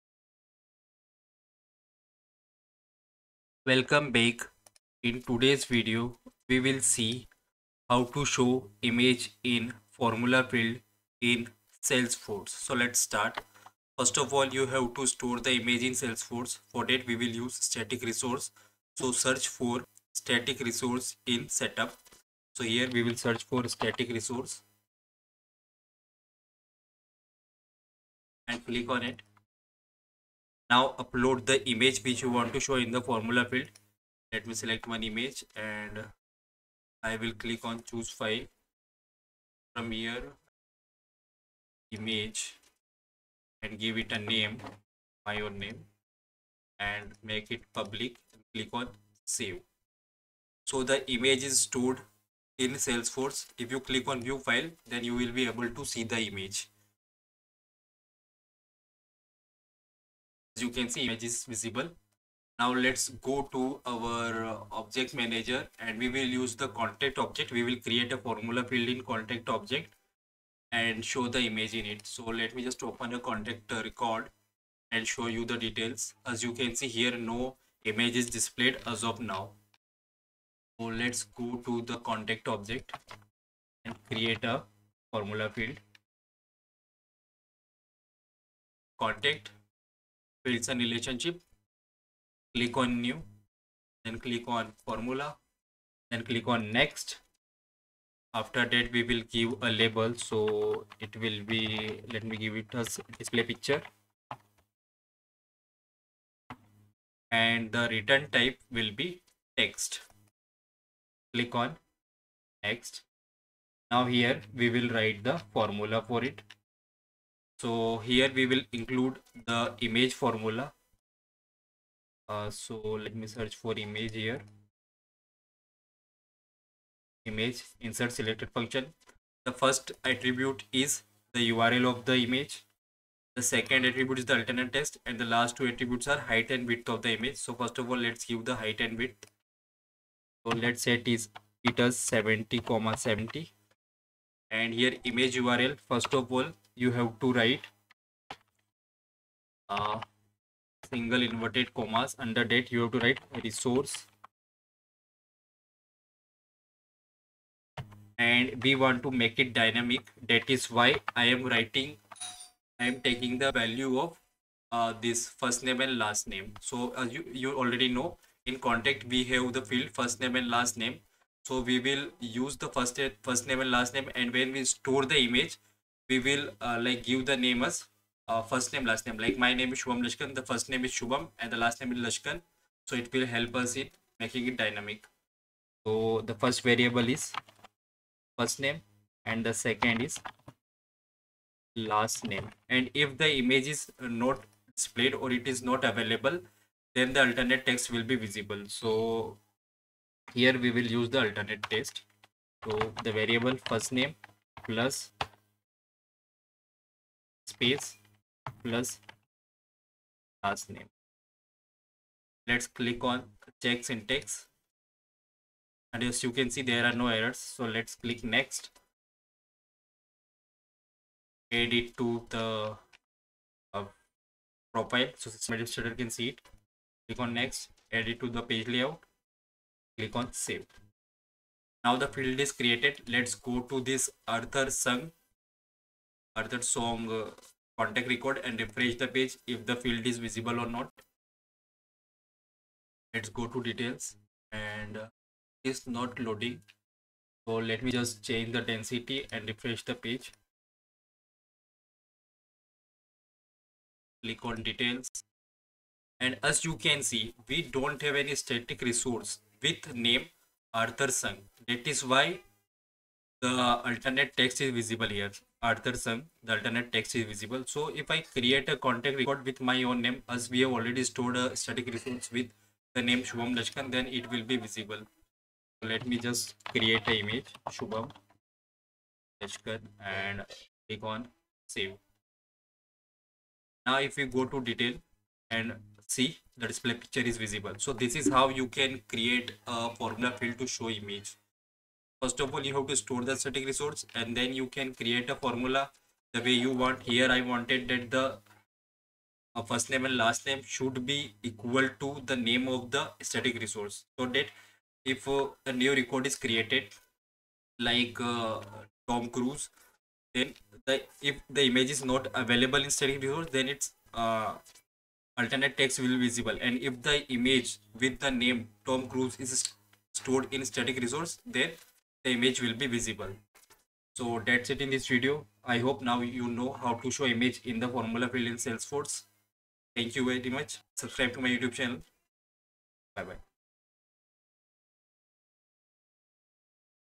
How to show image in formula field in salesforce? - Salesforce
In this video we will learn How to show image in formula field in salesforce?

Here is the link to the Salesforce Lightning Web Components for beginners course